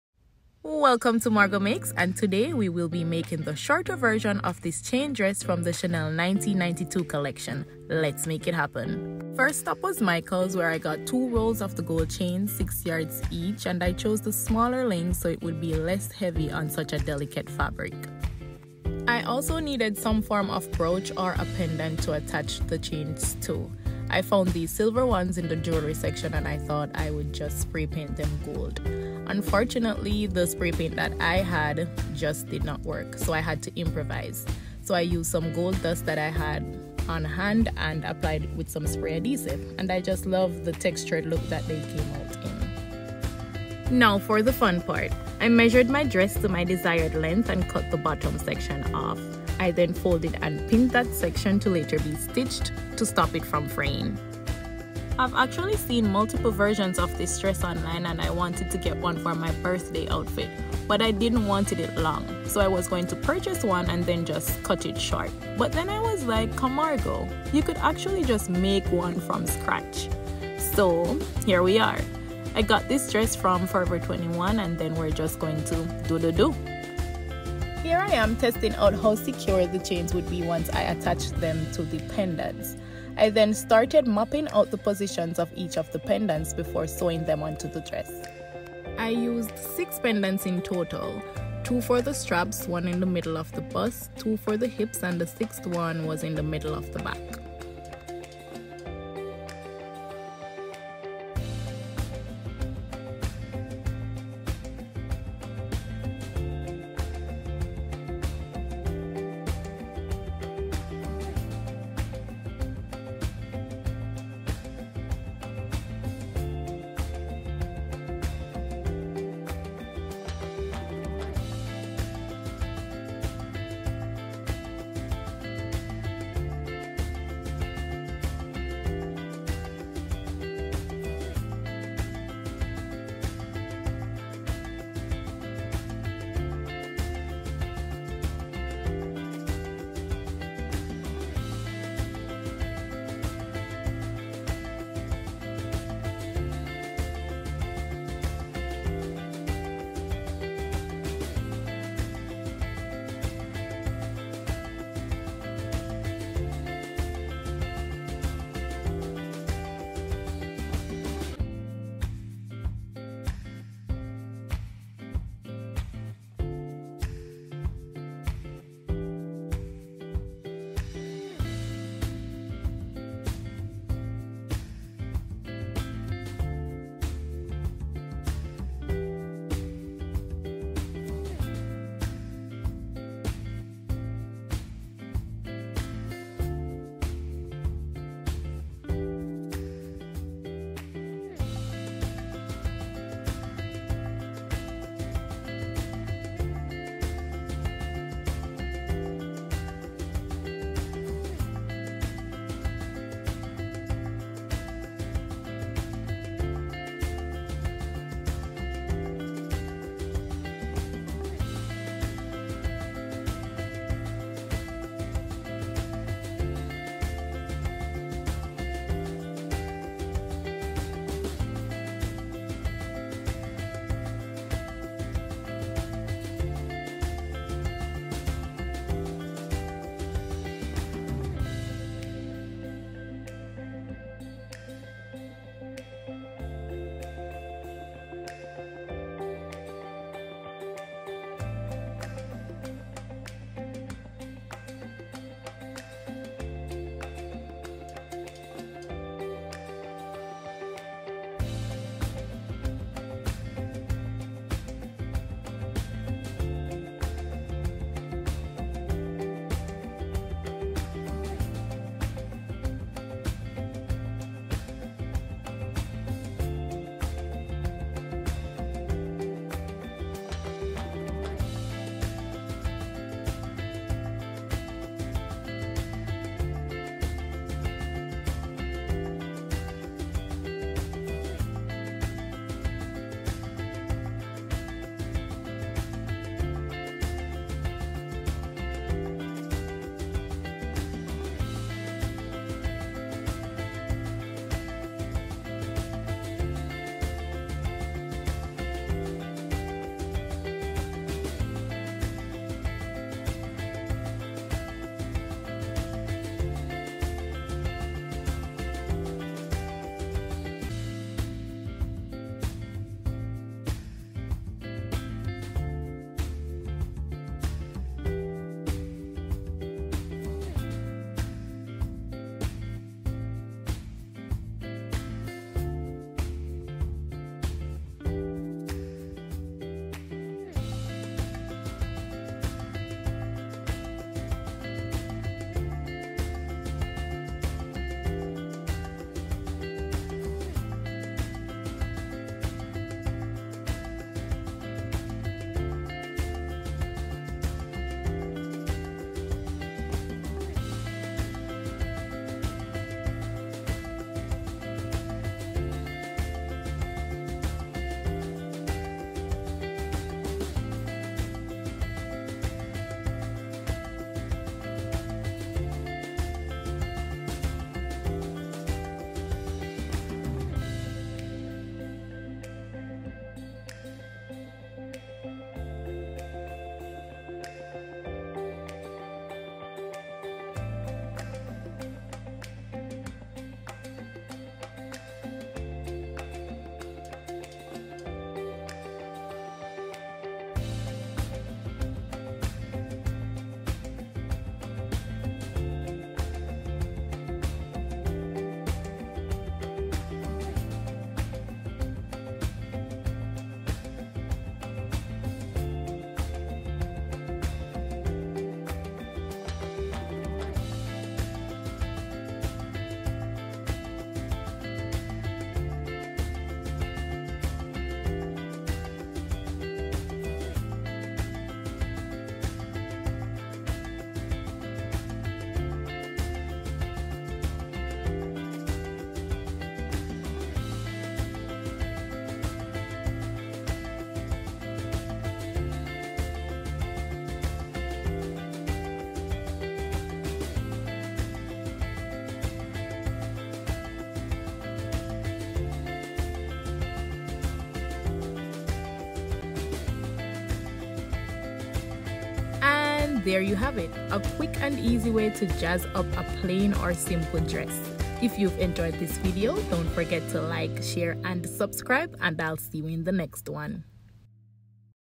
Recreating a Vintage Find: DIY Chain Dress Tutorial | Chanel 1992 Inspired
In this video, I'll be showing you how to create your own stunning Chanel 1992 chain dress with just a few basic sewing skills and materials. Using a black dress and gold chains, we'll be recreating the iconic look that's been seen on runways and red carpets for decades. I'll take you through each step of the process, from measuring and cutting the fabric to attaching the chains and finishing the dress. Whether you're a seasoned sewer or a beginner, this tutorial is perfect for anyone looking to add a touch of high fashion to their wardrobe. So grab your sewing machine, a needle and some thread, and let's MAKE it happen!